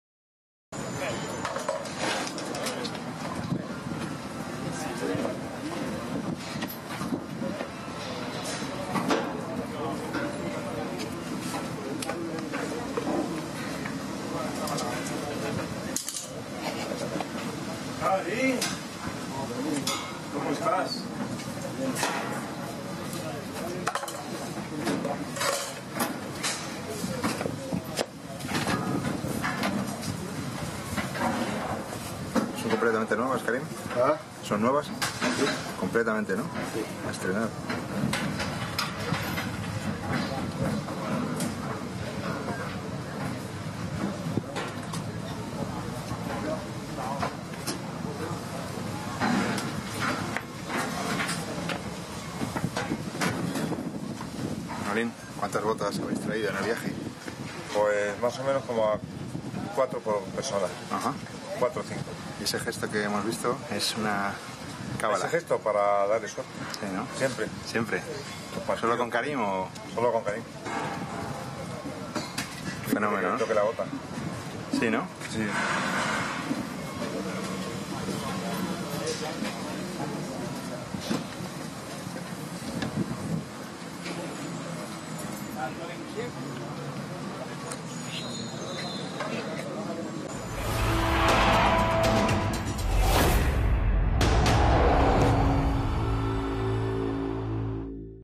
Karim Benzema's ritual before taking to the pitch
See how Real Madrid's French forward prepares himself before taking to the field of play.